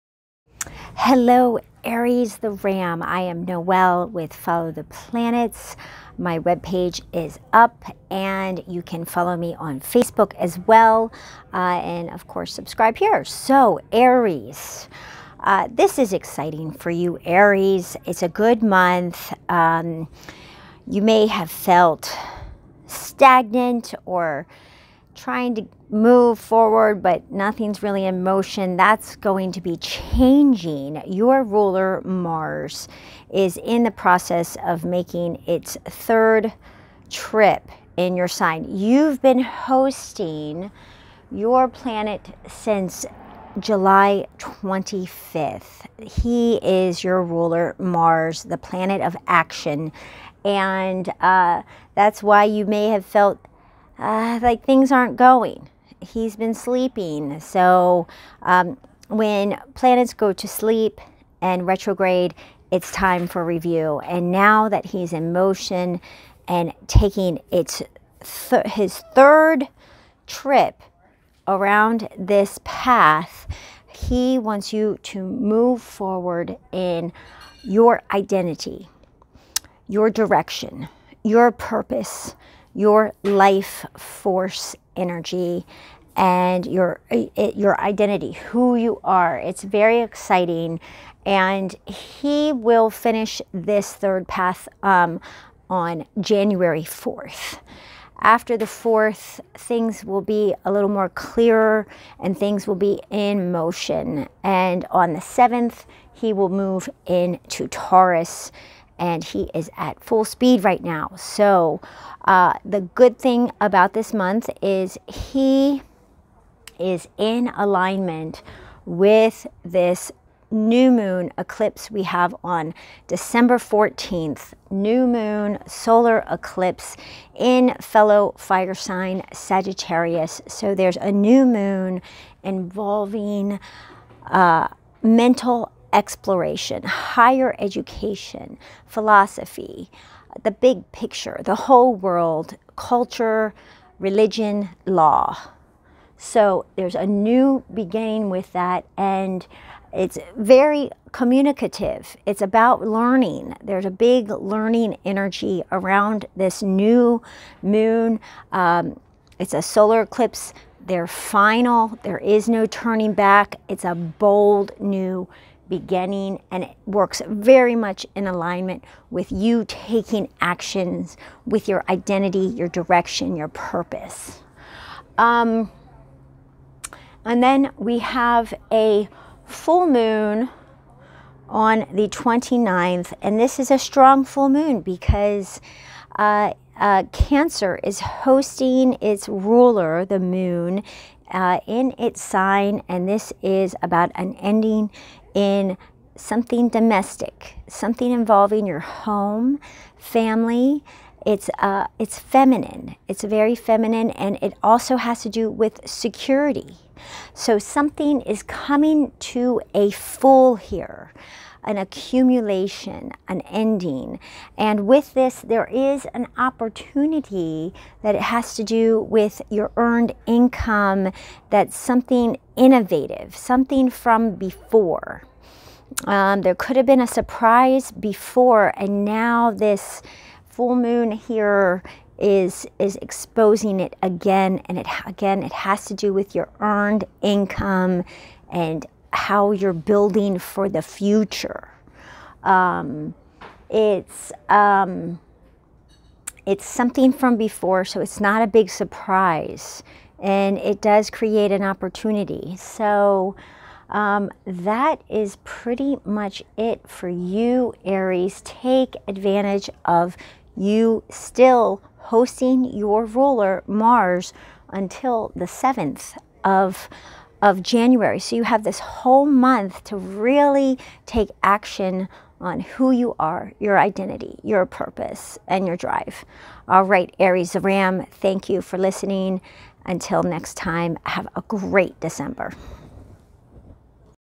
Aries Horoscope December 2020
Dear Aries the Ram, your Ruler Mars the Planet of Action, is in full motion after being retrograde for about 2 months.  You will continue to host your ruler through the month of December and he will move into Taurus on January 6th.  He is traveling over this path he’s traveled already 2 times before, this is his final time and he wants you to update your identity, your passion, your purpose, your direction, your life force with all that you’ve been reviewing and learning since July 25th, 2020.  After January 4th, things will be clear in your direction, your drive, your leadership...that is if you’ve taken the time to review these areas of yourself dear Aries.  If you journal, you are at an advantage, because you can look over your life’s events, thoughts, actions, non-actions, workouts, projects, leadership style since July 25th to review this journey you’ve been on and learn from it in order to create a new you!  If you’ve been following me dear Aries, I’ve been asking you to review these areas of your life, and now the time has come for you to take action in your learning lessons that make you a better Earthling...that’s what it's all about!